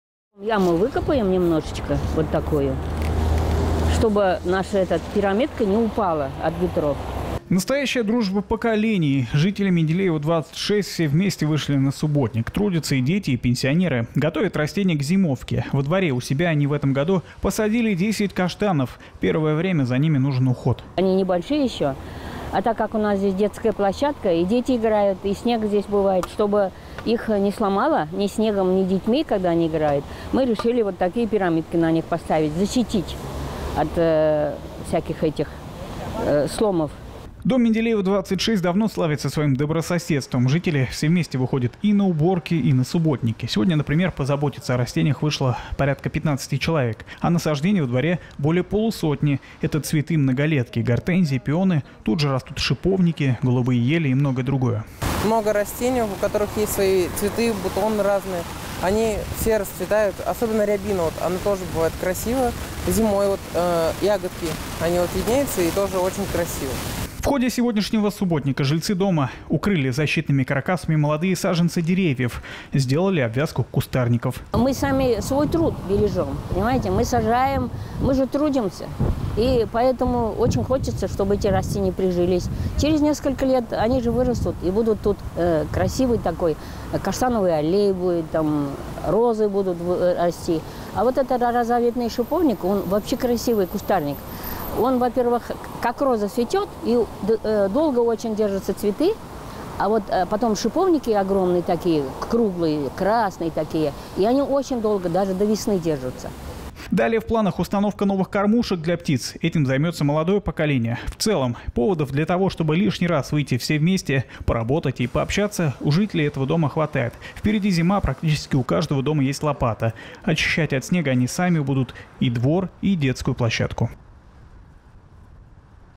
Жители нижнекамской многоэтажки показывают всем пример добрососедства
Дом № 26 по улице Менделеева в Нижнекамске известен своей сплоченностью.

Жители все вместе ухаживают за растениями, убирают придомовую территорию, добиваются обустройства детской площадки. В этот раз они все вместе подготовили кустарники и свежепосаженные каштаны к зимовке.